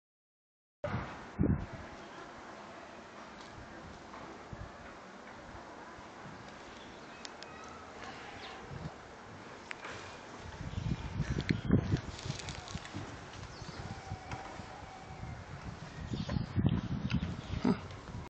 20110425 Ethiopia - Chalachew Gondar Tours 2.3gp
Castle of Emperor Fasilides (Fāsīladas) throne name ʿAlam Sagad, Ge'ez, "to whom the world bows")
Chalachew Bantiwalu (likes to called Charles) is a fantastic tour guide born & educated in Gondar. He has a an in-depth knowledge of local history & often contextualizes his narrative against world historic events.
Additionally he's a genuinely nice guy!